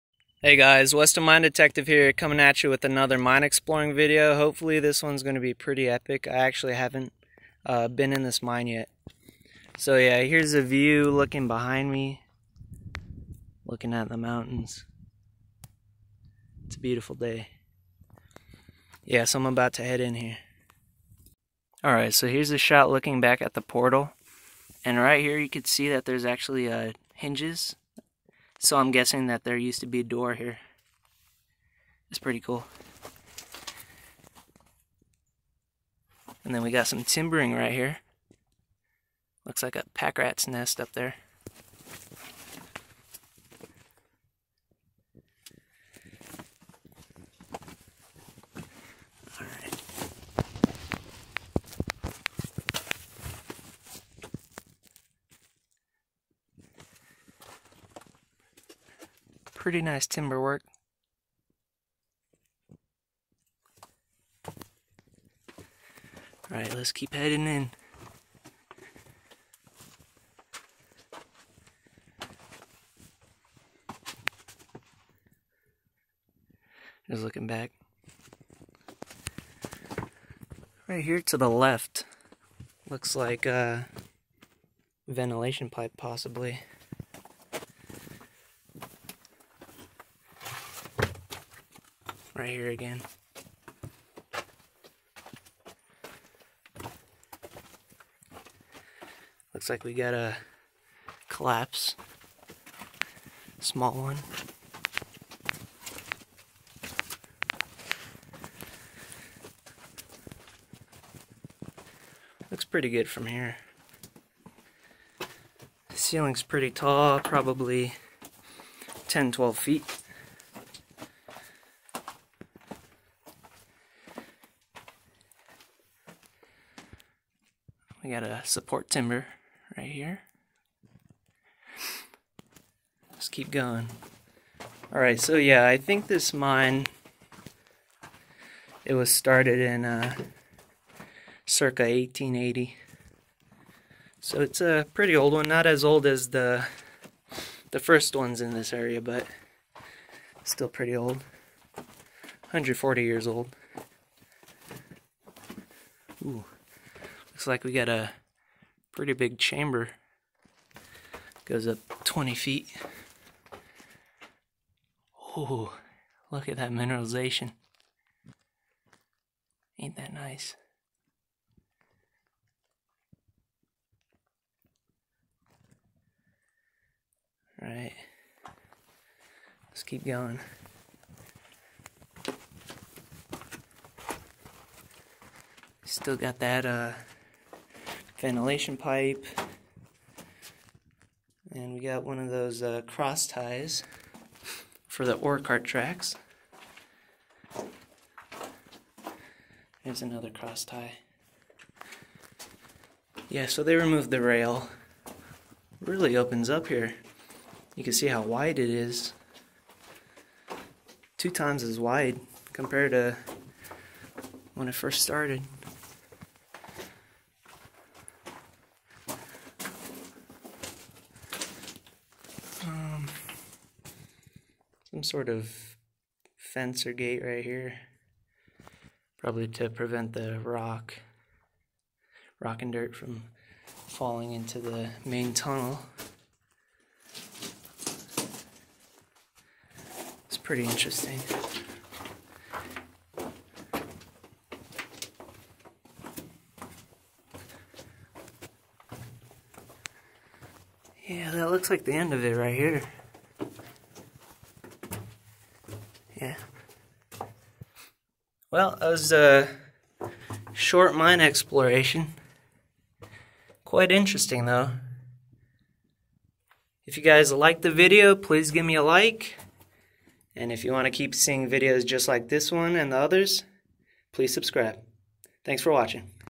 Exploring the Partially Collapsed Golden Gem Mine
Quick little explore of the Golden Gem mine. Started in 1880, this mine and others in the area produced millions of dollars of gold. Wasn't as extensive as I thought it would be, but it was a pretty quaint and interesting mine.